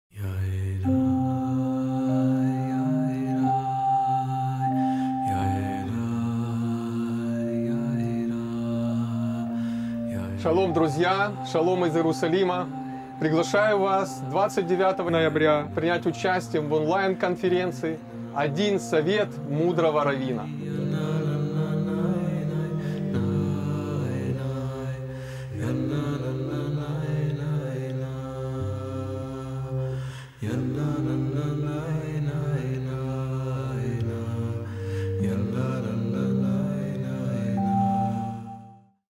Ариэль Ревах приглашение на онлайн конференцию "Один совет мудрого раввина"
29 ноября 2020 в 11:00 (по киевскому времени) приглашаем вас на онлайн конференцию «Один совет мудрого раввина» и главной темой в этот раз будет «Израиль - ключ к близким отношениям с Богом». Мессианские пастора и раввины из Украины, США и Израиля поделятся  с вами откровением Божьего Слова и свои личным опытом касательно Израиля, молитвы и близких отношений с Богом.
Спикеры конференции:
- Валентин Свионтэк ( ЕМО «Ор хаМашиах» Одесса, Украина)
- Инна Георг ( ЕМО «Бэйт Симха» Черновцы, Украина)
- Эзра Бенджамин  (служение «Jewish Voice» Феникс, США)
- Ариэль Ревах (ЕМО «Еуд Исраэль» Рамле, Израиль)
- Израель Почтарь («Байт Алель» Ашдод, Израиль)
Почётный гость конференции:
 Сергей Шидловский (основатель «Международного молитвенного движения Искателей Бога»)
Организаторы конференции:
- Миссия JPI (Jewish Partner Initiative)
- Ш.М.О.Т. ( Школа Мессианского обучения Талмедим) г.Одесса, Украина
Информационная поддержка:
БогТВ